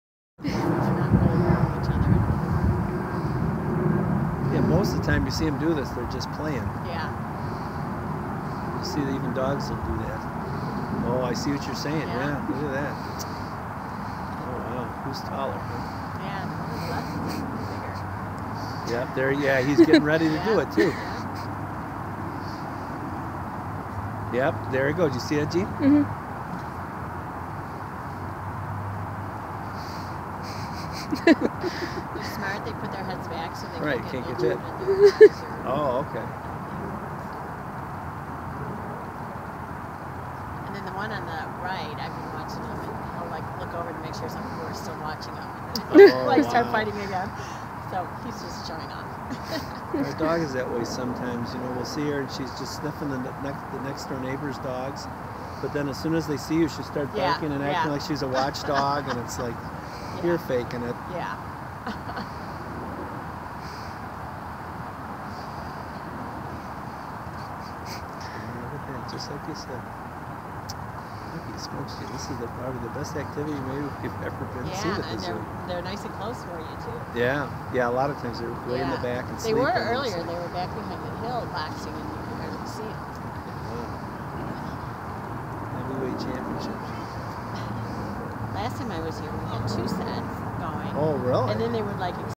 Kangaroos Boxing at the Detroit Zoo
It was really interesting to see this at the zoo with my dad.  They were obviously looking our attention because they kept looking back to make sure we were watching.